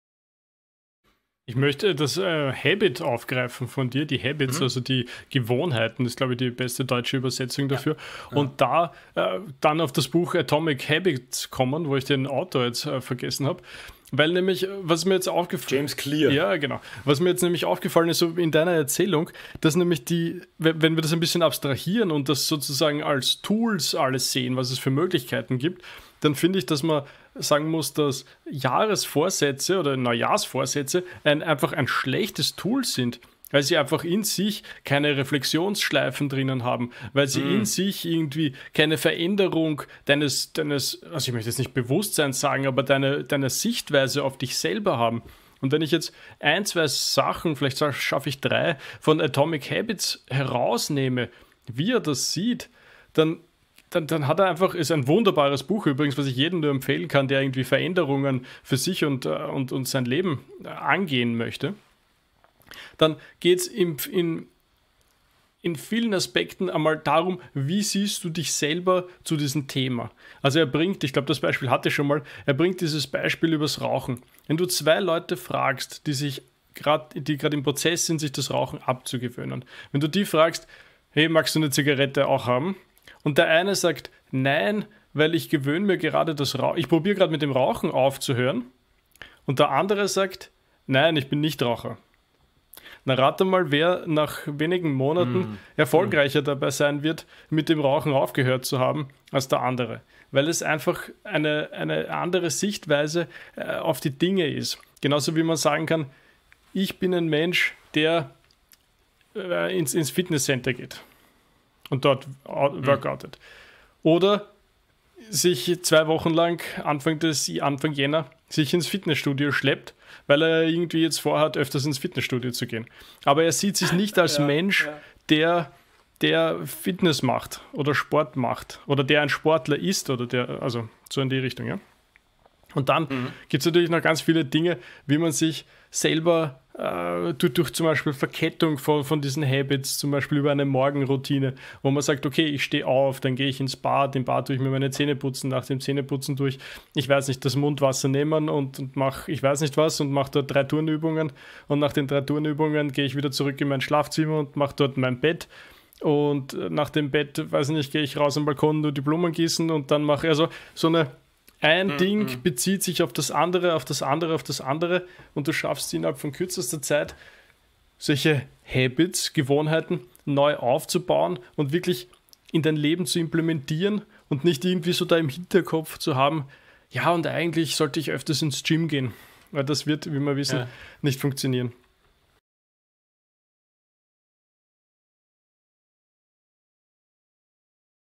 Wie baut man erfolgreich Gewohnheiten auf?
In diesem kurzen Clip spricht Flo über die richtigen Kommunikations- bzw. Mindset-Methoden für die Entwicklung von Gewohnheiten.